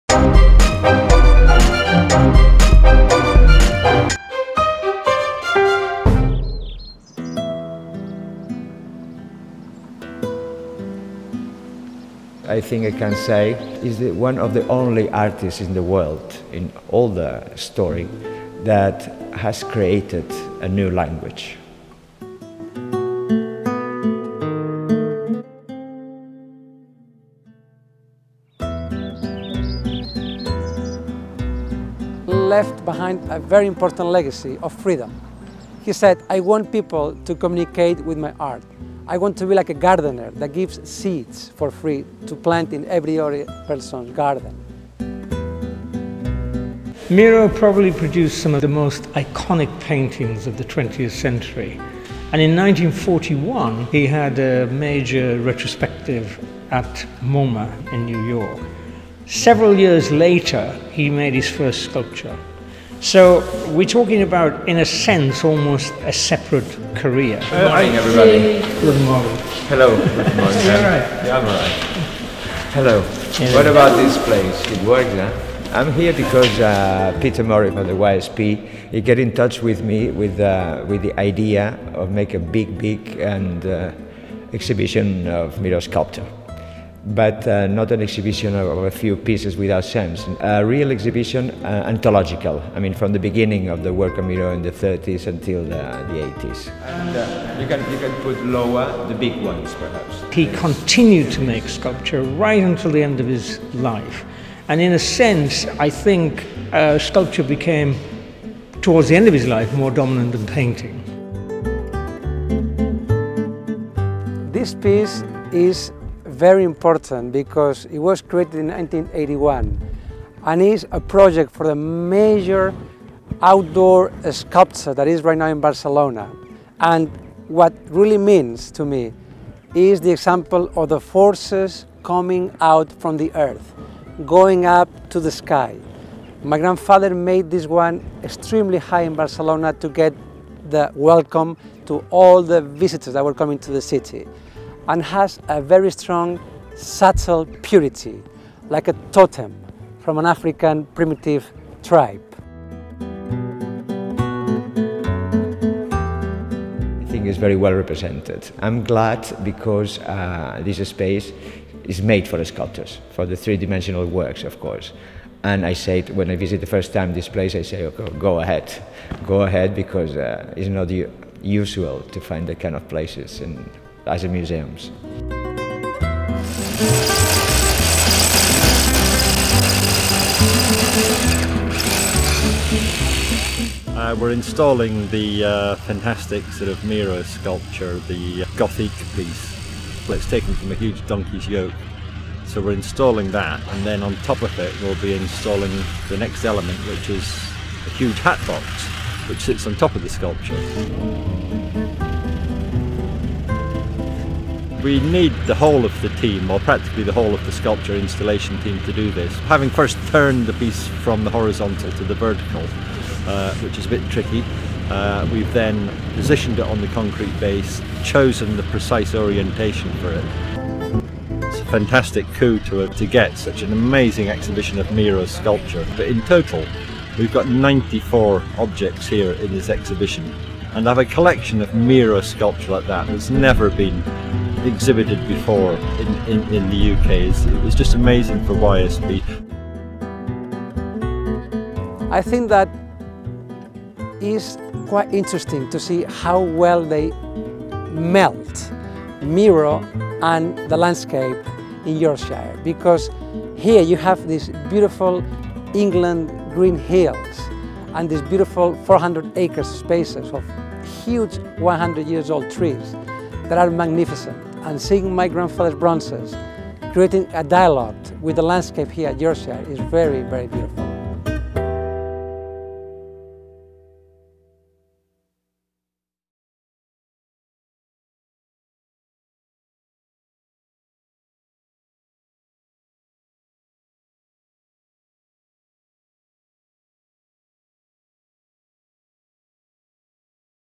Art in Yorkshire - Miro:Sculptor at Yorkshire Sculpture Park
Art in Yorkshire takes a look Miro:Sculptor,  the first major British exhibition of Miro's sculpture, set in the stunning landscape of the Yorkshire Sculpture Park.